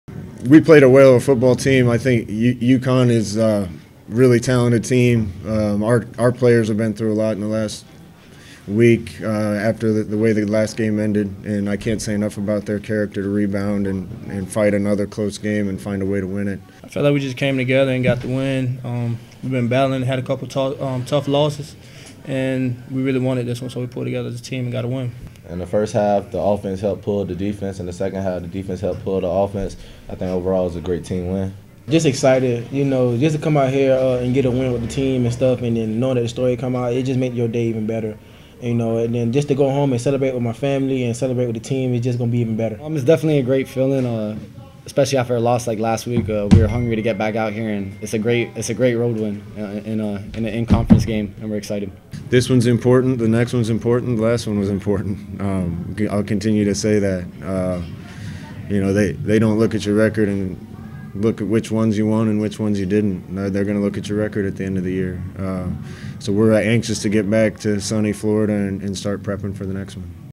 RECAP: UCF Football at UConn (10-23-16)
Hear what the Knights had to say after their 24-16 win against the UConn Huskies.